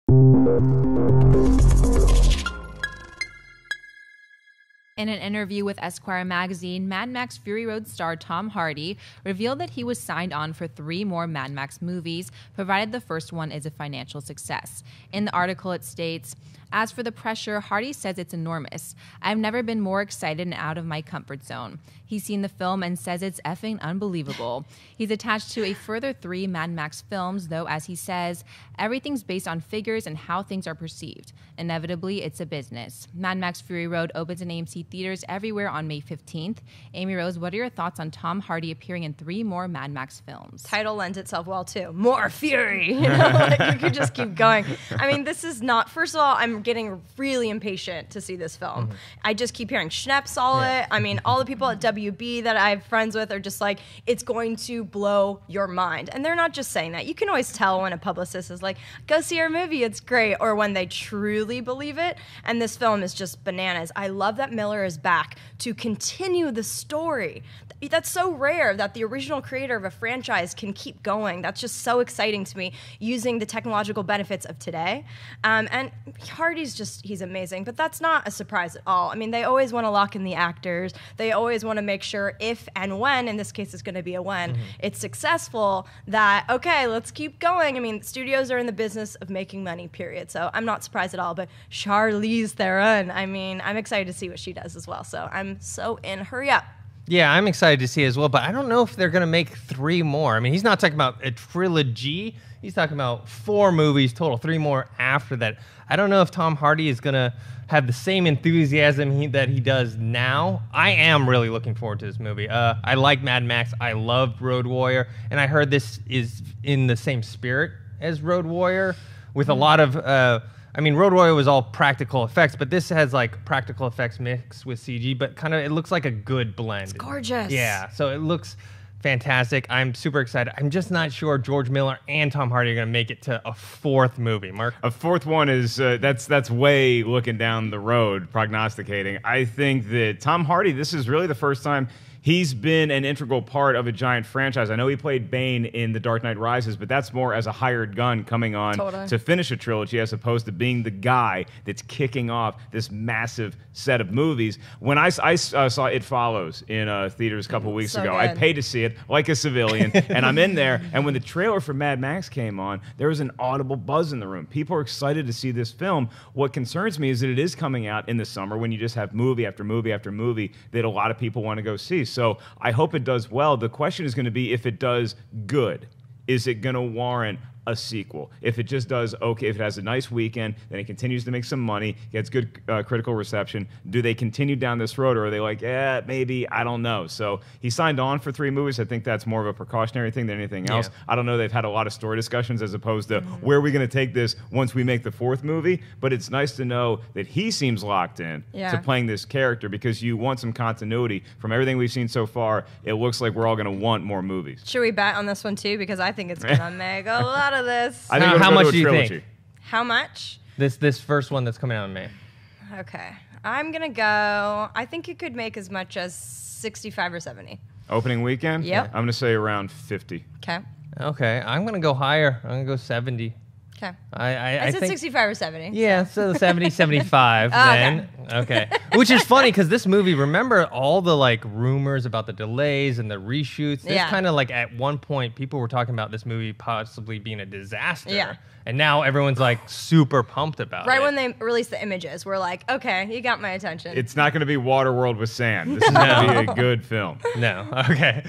3 More MAD MAX Sequels - AMC Movie News
In an interview with Esquire magazine, Mad Max: Fury Road star Tom Hardy revealed that he was signed on for three more Mad Max movies provided the first one is a financial success. In the article it states:
As for the pressure, Hardy says it’s enormous. “I’ve never been more excited and out of my comfort zone.” He’s seen the film, and says it’s “f-ing unbelievable”. He’s attached to a further three Mad Max films, though as he says, “Everything’s based on figures and how things are perceived. Inevitably it’s a business.”
Mad Max Fury Road opens in AMC Theatres everywhere on May 15th.